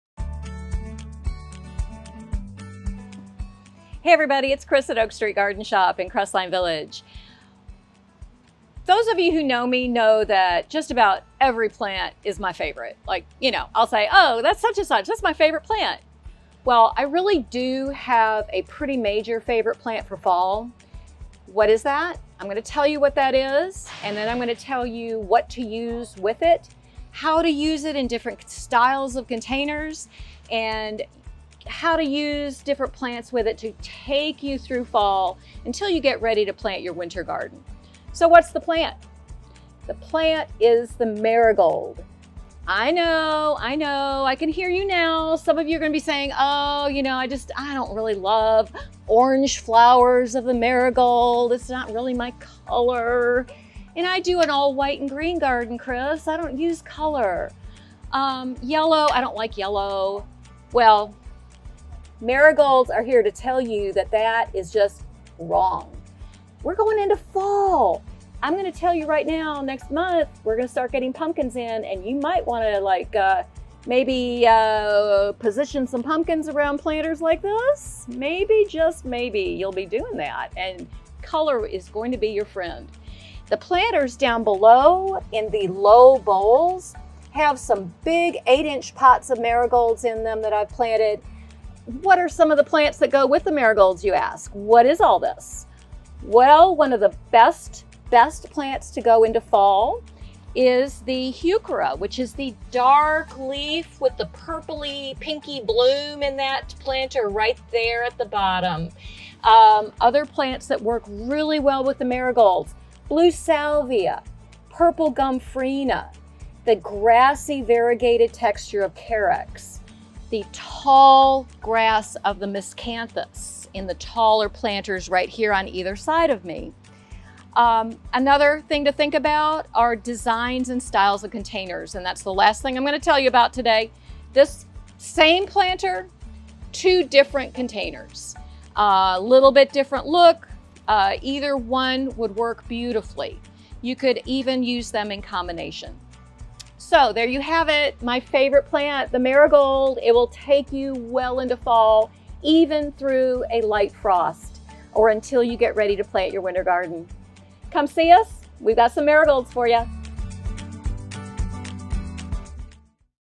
Our Minds are on Marigolds!
It’s no secret one of our favorite plants for fall is the marigold! Don’t let the bright colors scare you away, we’ll help you build a great arrangement to work in your space. Come see these bright blooms for yourself!